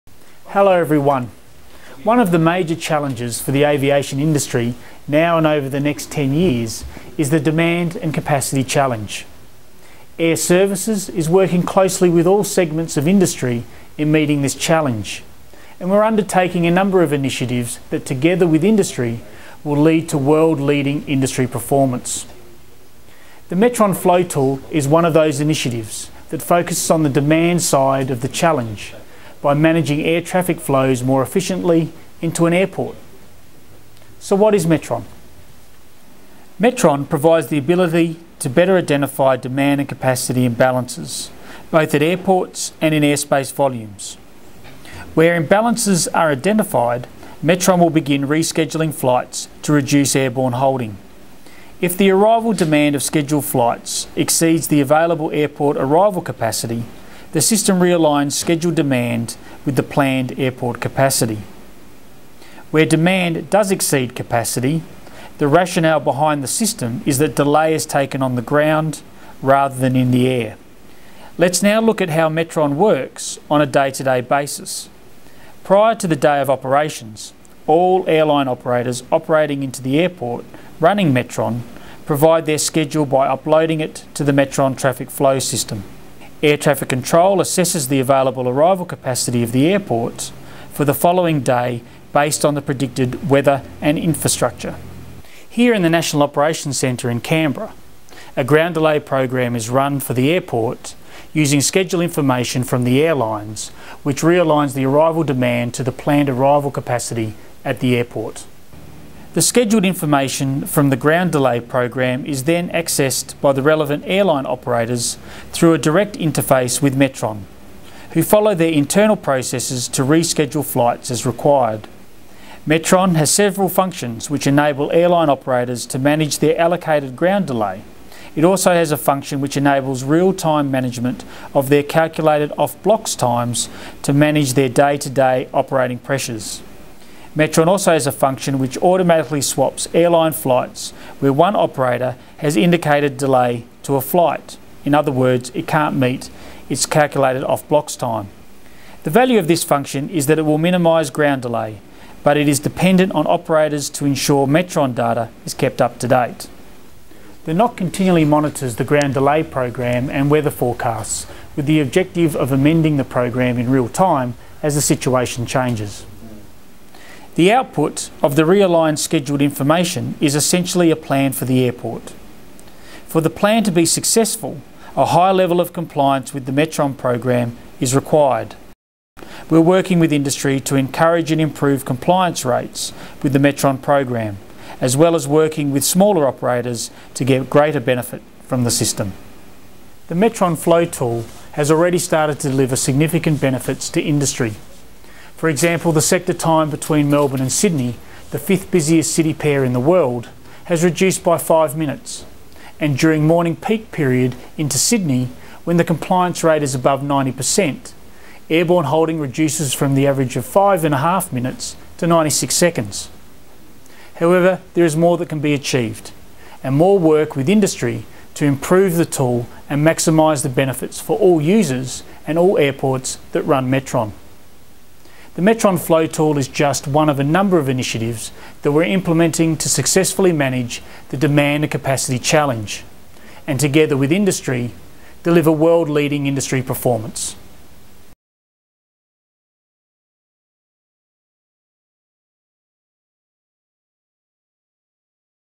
Airservices introduces Metron Traffic Flow
The first stage of a new collaborative approach to industry-wide air traffic management in Australia has seen positive results for airlines, with an average reduction of five minutes flight time per flight on the Melbourne-Sydney city pair.

Software developed by Virginia-based Metron Aviation is behind the new approach to information sharing and air traffic management.

Metron Traffic Flow provides airlines with collaborative decision making features that enable them to manage their fleets to ensure optimum use of available capacity within and across multiple airports to achieve their business goals and improve operational efficiency.

The Metron Traffic Flow project is a key enabler for Airservices as it works towards its vision of 'connecting the Australian aviation industry to deliver world class industry performance' for airlines, airports and the traveling public.